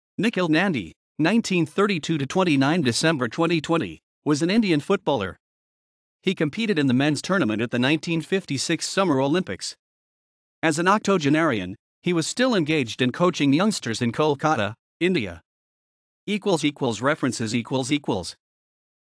Nikhil Nandy
Nikhil Nandy (1932 – 29 December 2020) was an Indian footballer. He competed in the men\s